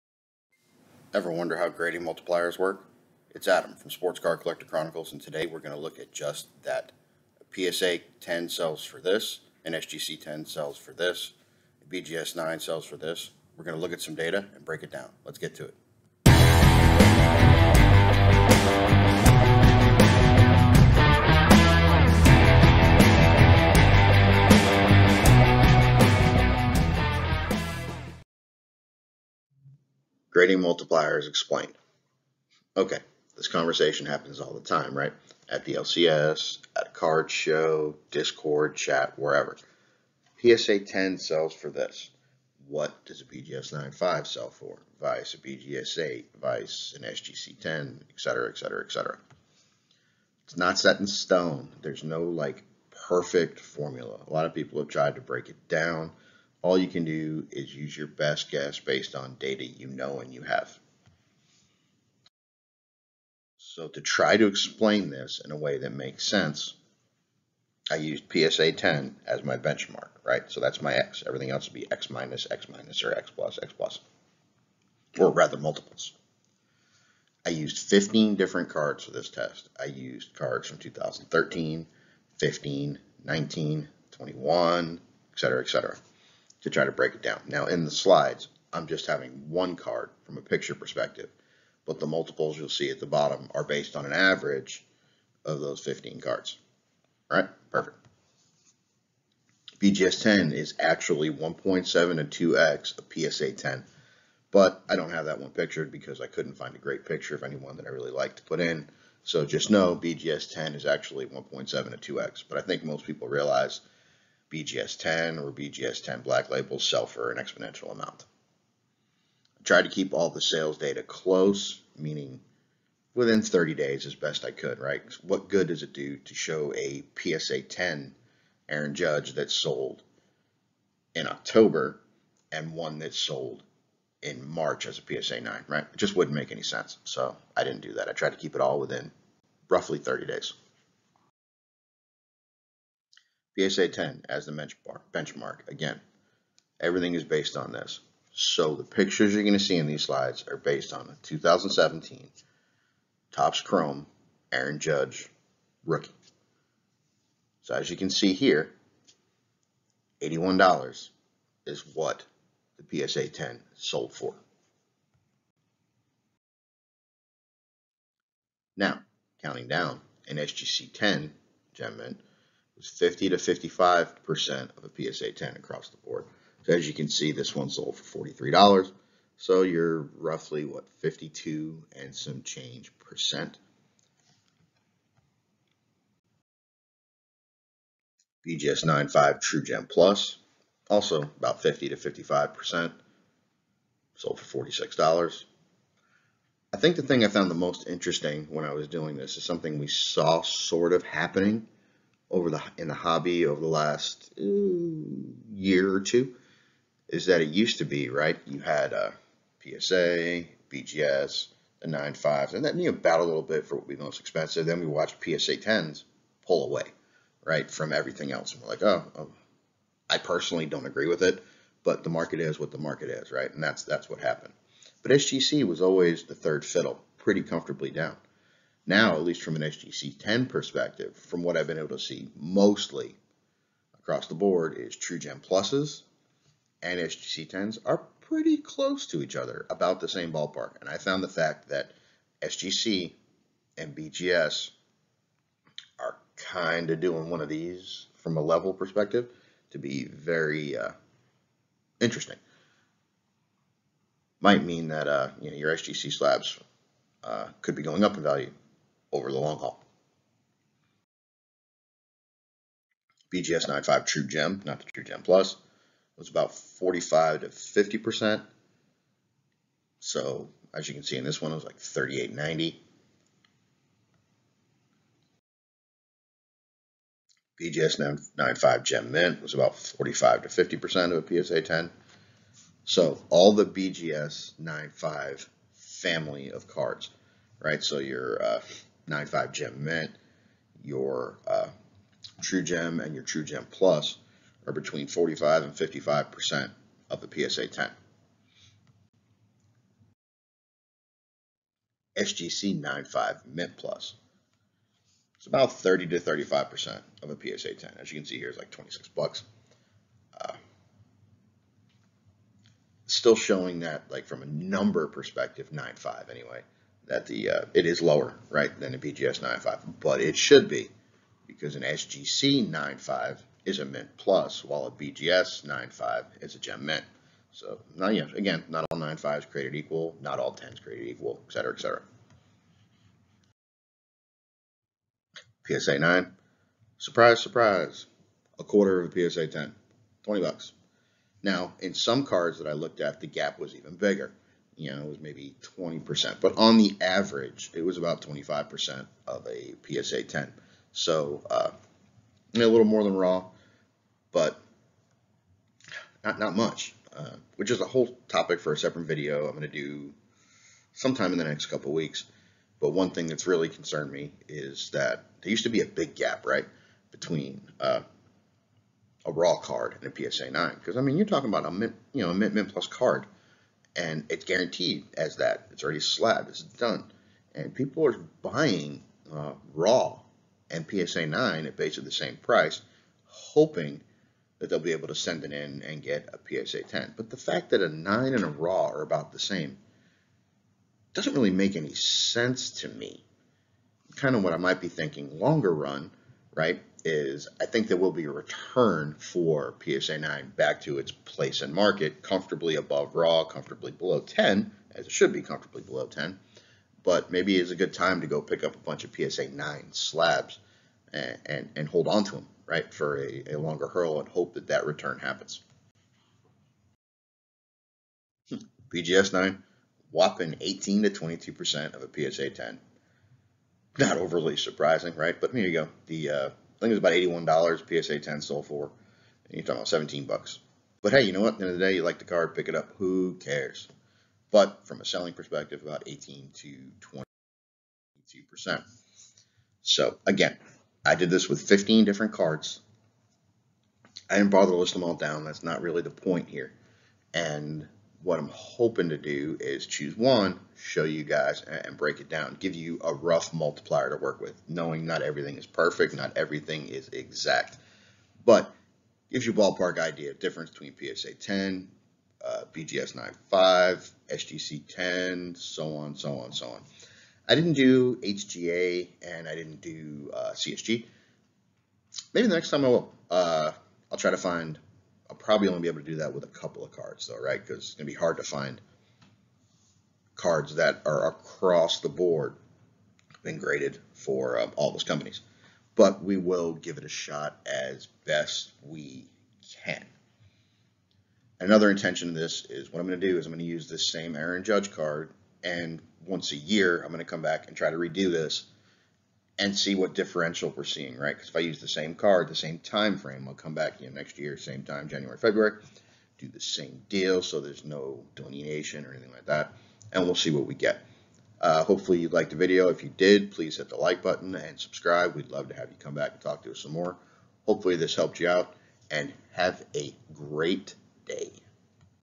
PSA 10 sell prices are INSANE compared to other grades, really?
What are the price differences between BGS graded cards, PSA graded cards, and SGC graded cards? In this video I de-mystify the correlation between sale values related to PSA, BGS, and SGC slabs as well as a few interesting tidbits and market trends over the past couple of years. I hope you enjoy.